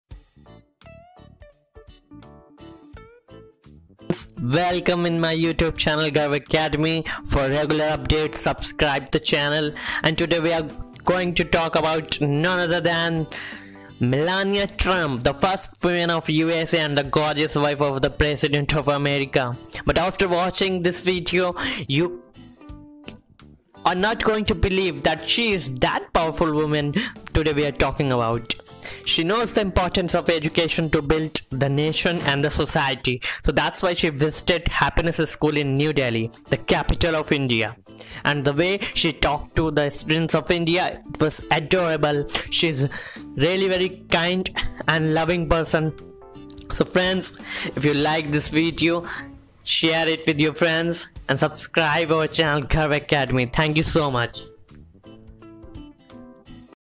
Melania Trump || मेलानिया ट्रम्प
Aloha ! In this video we are going to talk about the First women of America .